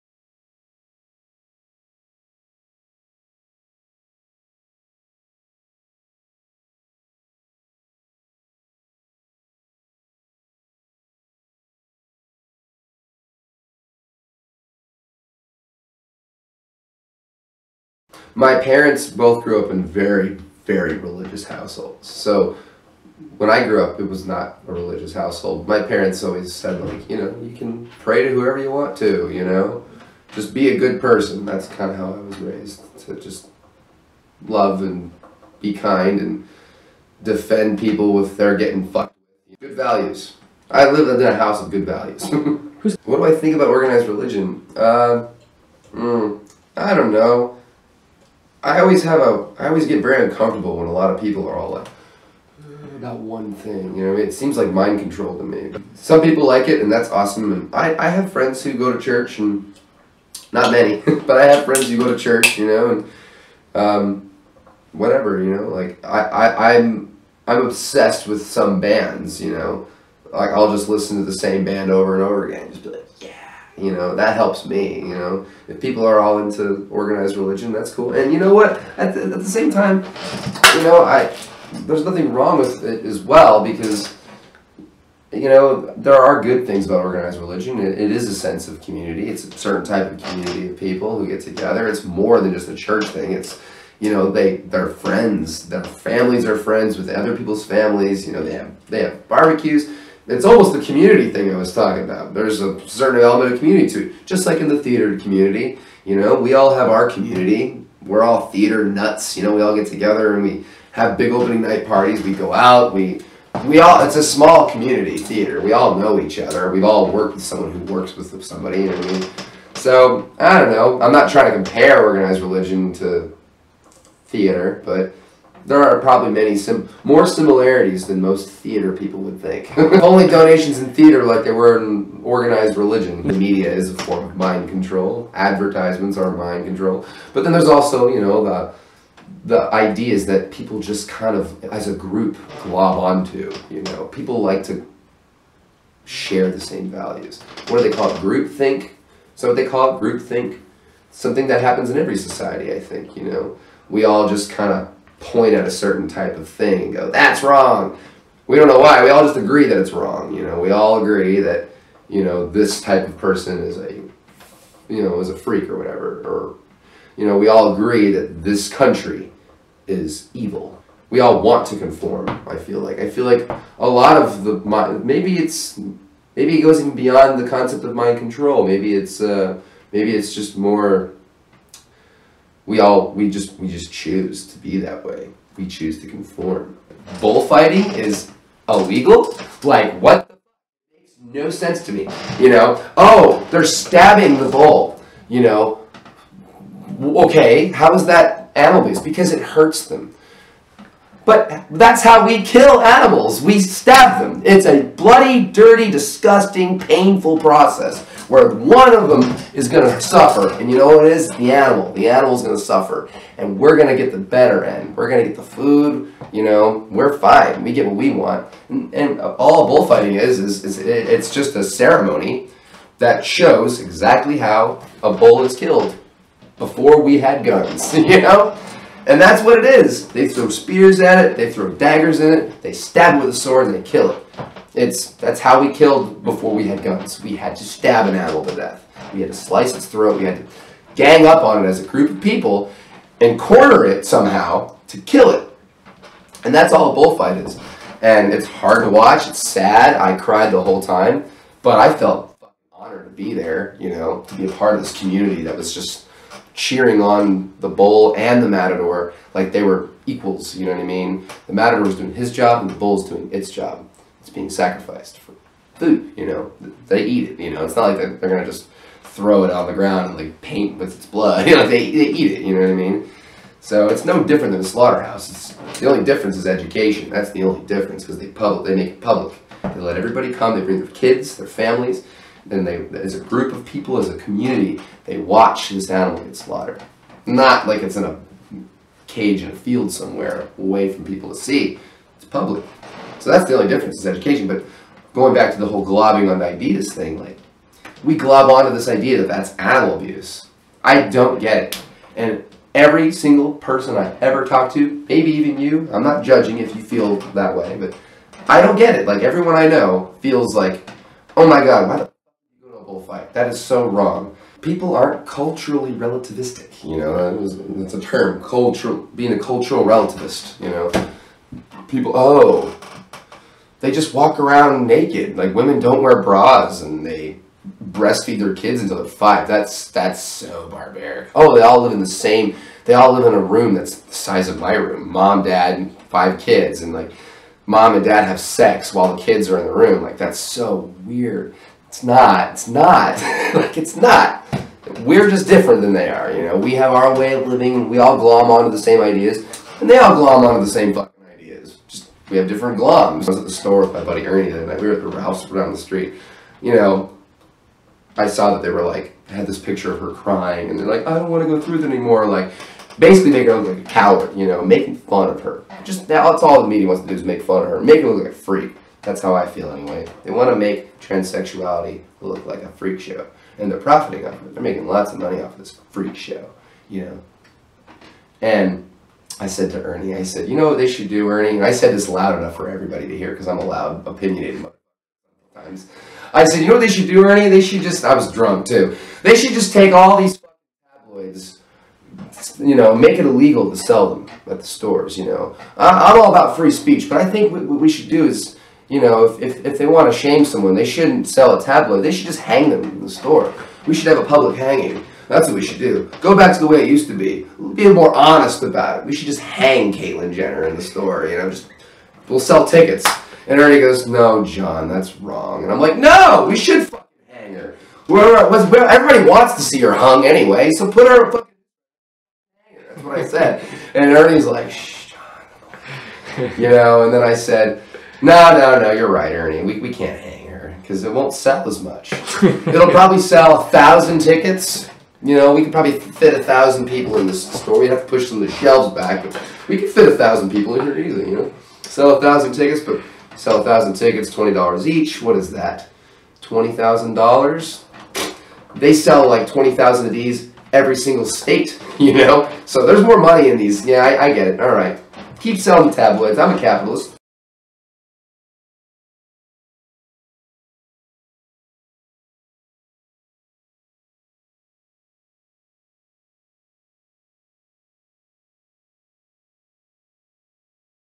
Jon Huckaby Interview - Part 8 - Religion & Ceremony - New Fortune Theatre Company
New Fortune Interview Series

Jon Huckaby - Stage Manager - Part 8 - Religion & Ceremony

Jon talks about religion, bullfighting and Caitlyn Jenner.